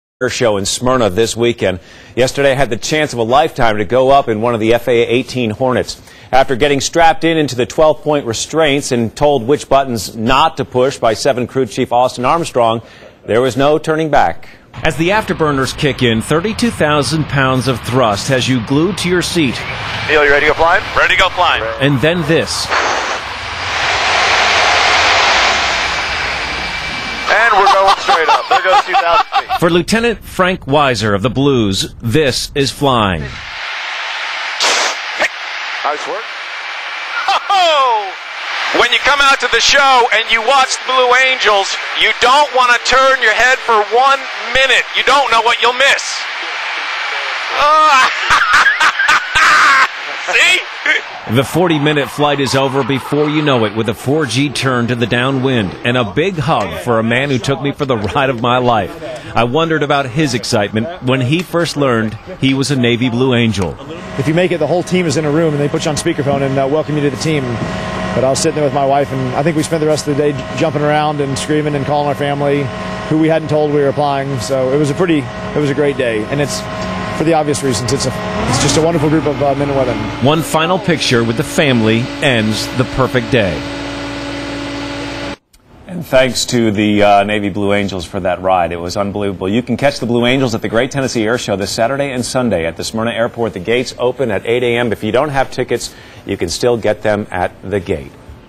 Blue Angels Orne
Blue Angels visit Nashville and take reporter for a ride.  At the control Lt. Frank Weisser aka "Walleye".  April 2008.